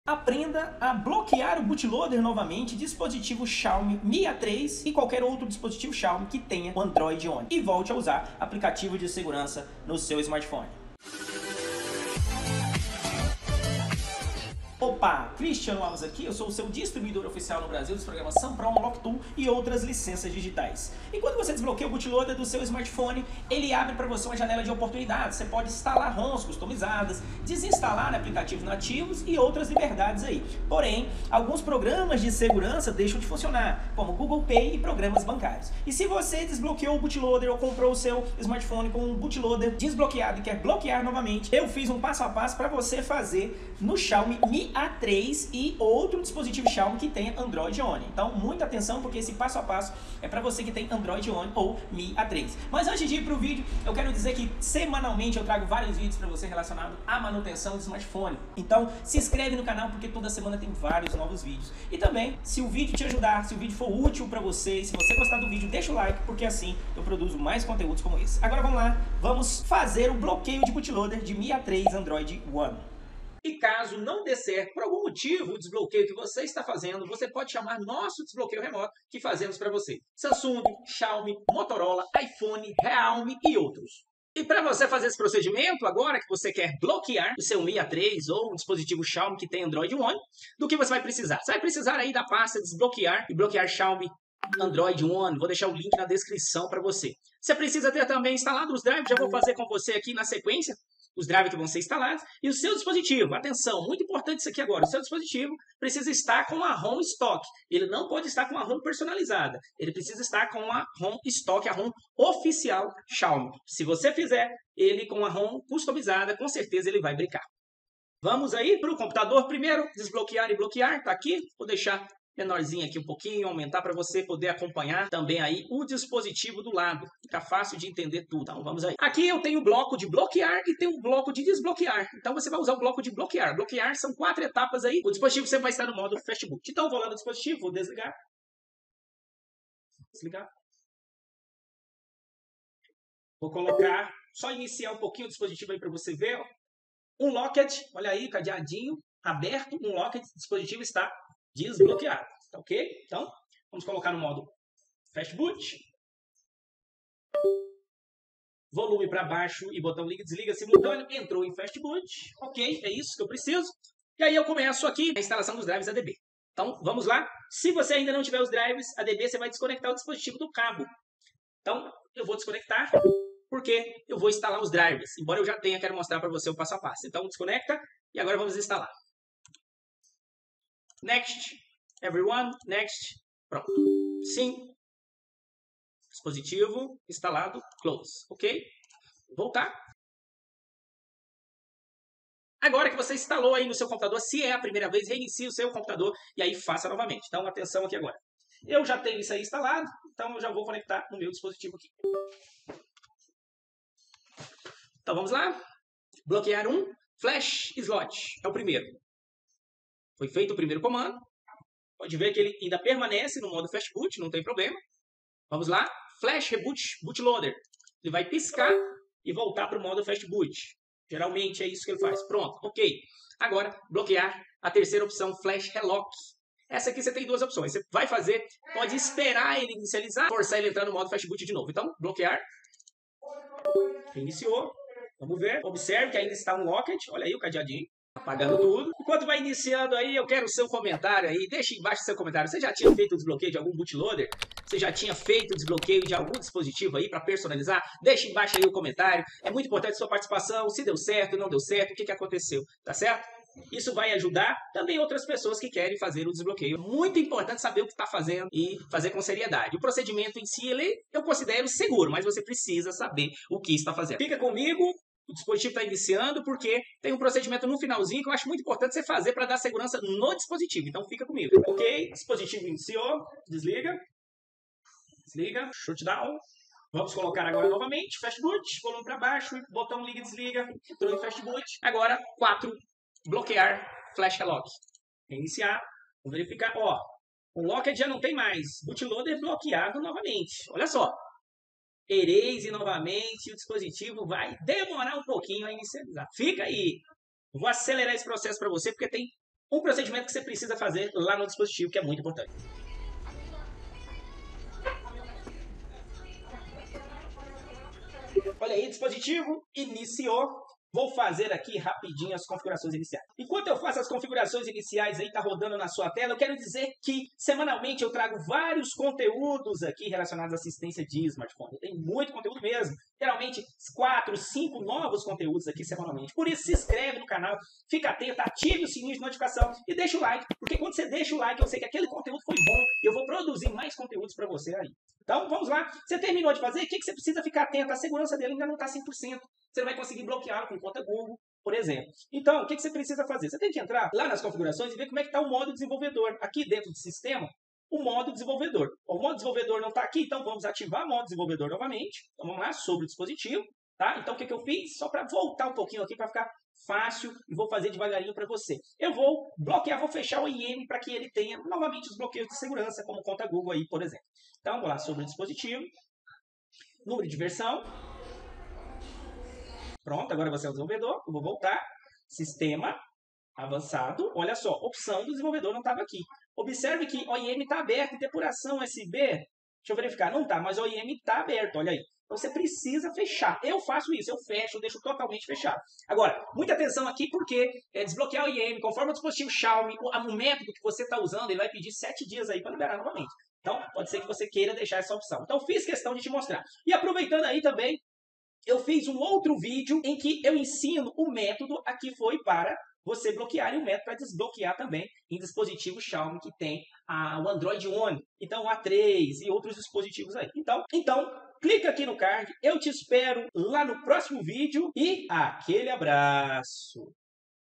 Como Bloquear Bootloader Xiaomi Mi A3 🔒
Aprenda a Bloquear o Bootoloader de Xiaomi MI A3 com Android One

⚠️ ISENÇÃO DE RESPONSABILIDADE:
📞 Fale Comigo e Tire Suas Dúvidas:‍